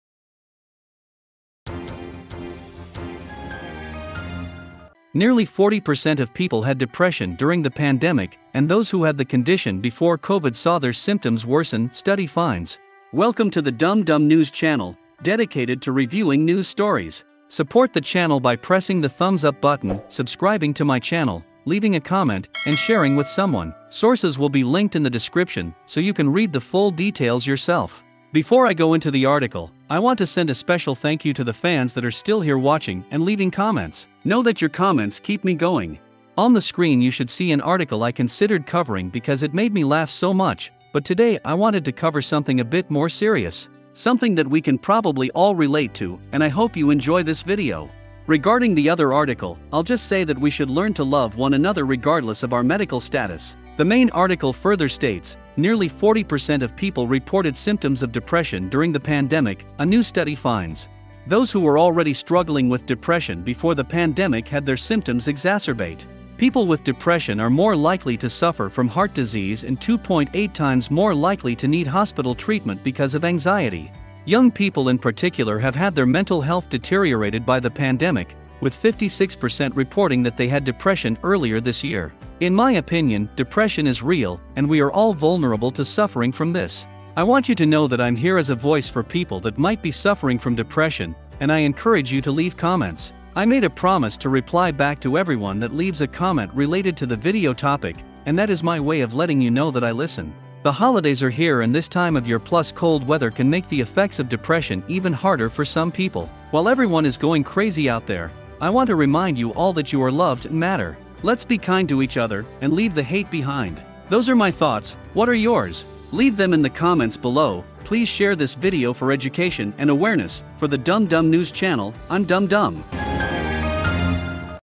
Nearly 40% of People Had Depression During The Last 1 Year Due To This Major Event.
Dum Dum

✭ Captioning ✭
News broadcaster Dum Dum, “Hi, I'm your host Dum Dum and welcome to the Dum Dum News Channel. In today's news Nearly 40% of People Had Depression During The Last 1 Year Due To This Major Event. "